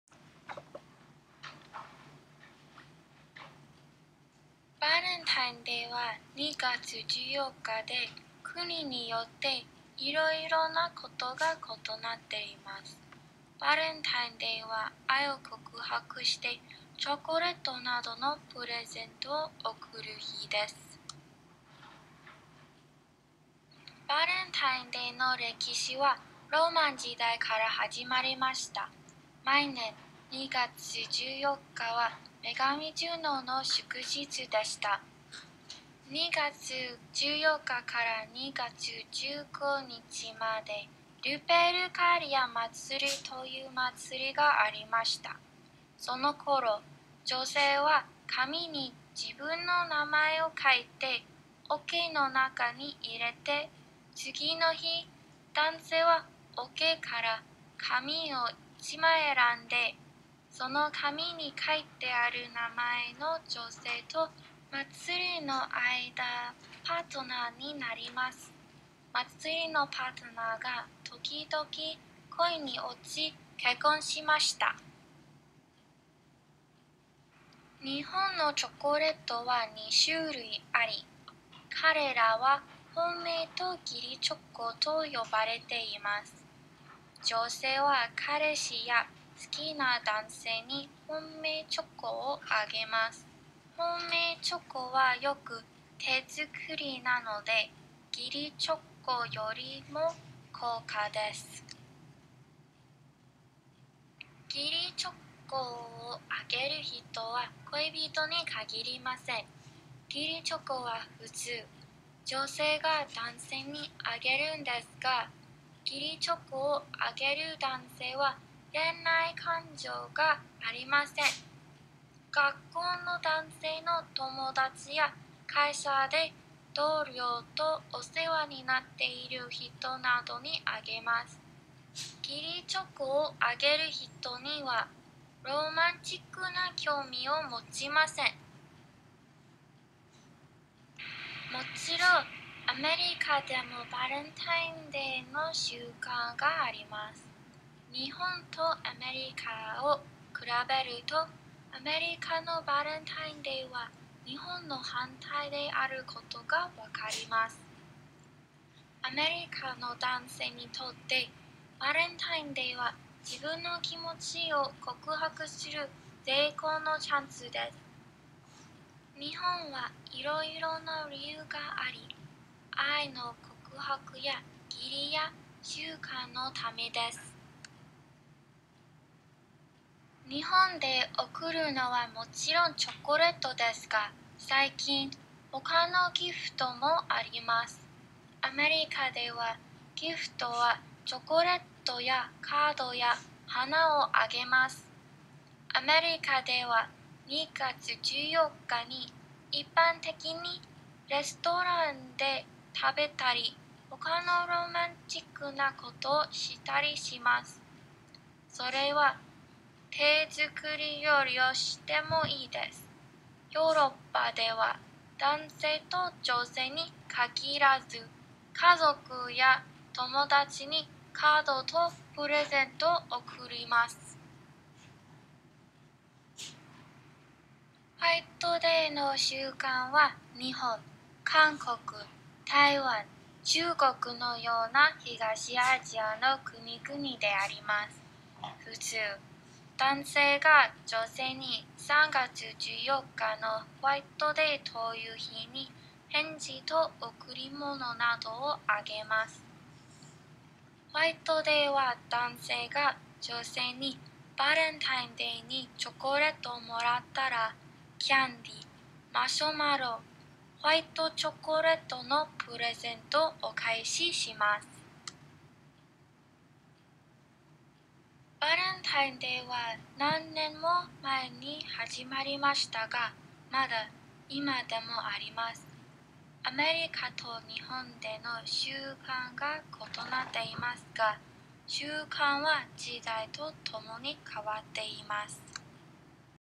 バレンタインデー by ルー
Valentines ay (JOSHU, UT Austin, Spring 2012 )
This video was created by one of the students of Japanese at the University of Texas at Austin for his/her project to introduce Japanese culture.